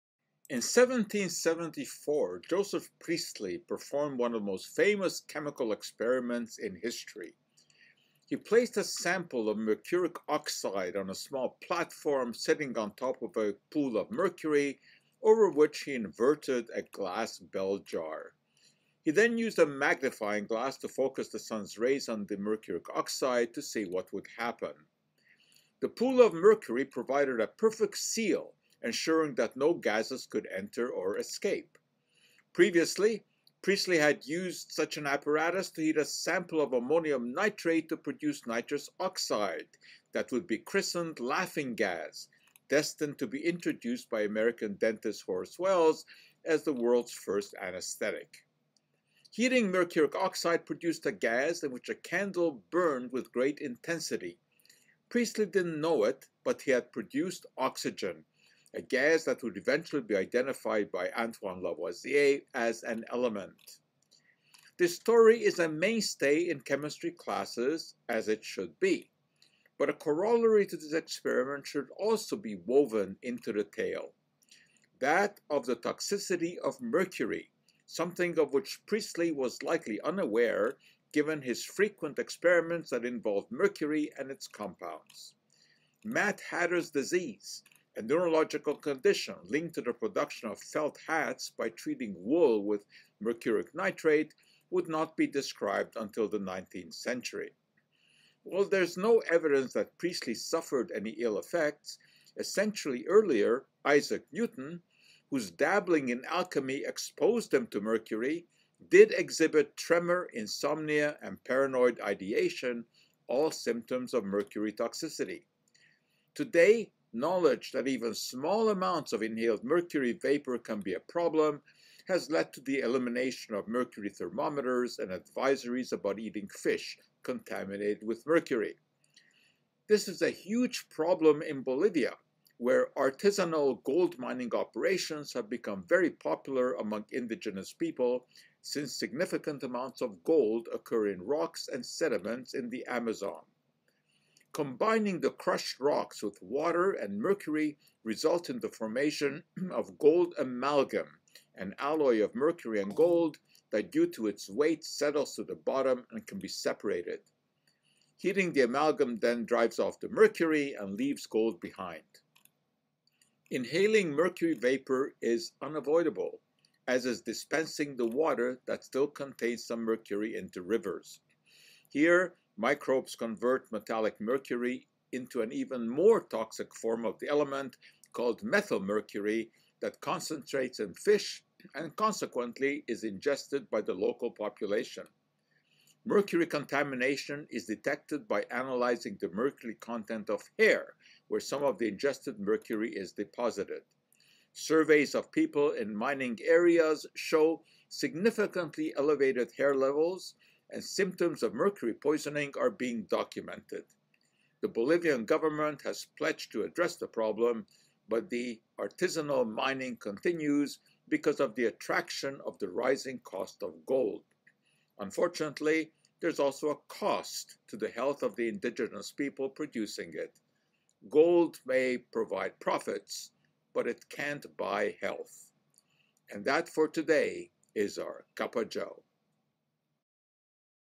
Cup o'Joe-Mercurial concerns
In 1774 Joseph Priestley performed one of the most famous chemical experiments in history. He placed a sample of mercuric oxide on a small platform sitting on top of a pool of mercury over which he inverted a glass bell jar. He then used a magnifying glass to focus the sun’s rays on the mercuric oxide to see what would happen. The pool of mercury provided a perfect seal ensuring that no gases could enter or escape. Previously Priestley had used the apparatus to heat a sample of ammonium nitrate to produce nitrous oxide that would be christened “laughing gas,” destined to be introduced by American dentist Horace Wells as the world’s first anesthetic. Heating mercuric oxide produced a gas in which a candle burned with great intensity. Priestley didn’t know it, but he had produced oxygen, a gas that would eventually be identified by Antoine Lavoisier as an element.
This story is a mainstay in chemistry classes, as it should be. But a corollary to this experiment should also be woven into the tale. That of the toxicity of mercury, something of which Priestley was likely unaware given his frequent experiments that involved mercury and its compounds. “Mad hatter’s disease,” a neurological condition linked to the production of felt hats by treating wool with mercuric nitrate would not be described until the 19th century. While there is no evidence that Priestley suffered any ill effects, a century earlier, Isaac Newton, whose dabbling in alchemy exposed him to mercury, did exhibit tremor, insomnia and paranoid ideation, all symptoms of mercury toxicity. Today, knowledge that even small amounts of inhaled mercury vapour can be a problem has led to the elimination of mercury thermometers and advisories about eating fish contaminated with mercury.
This is a huge problem in Bolivia where “artisanal” gold mining operations have become very popular among indigenous people since significant amounts of gold occur in rocks and sediments in the Amazon. Combining the crushed rocks with water and mercury results in the formation of gold “amalgam,” an alloy of mercury and gold that due to its weight settles to the bottom and can be separated. Heating the amalgam then drives off the mercury and leaves gold behind.
Inhaling mercury vapour is unavoidable, as is dispensing the water that still contains some mercury into rivers. Here, microbes convert metallic mercury into an even more toxic form called methyl mercury that concentrates in fish and consequently is ingested by the local population. Mercury contamination is detected by analysing the mercury content of hair where some of the ingested mercury is deposited. Surveys of people in the mining areas show significantly elevated hair levels and symptoms of mercury poisoning are being documented. The Bolivian government has pledged to address the problem, but the artisanal mining continues because of the attraction of the rising cost of gold. Unfortunately, there is also a cost to the health of the indigenous people producing it. Gold my bring profits but it can’t buy health.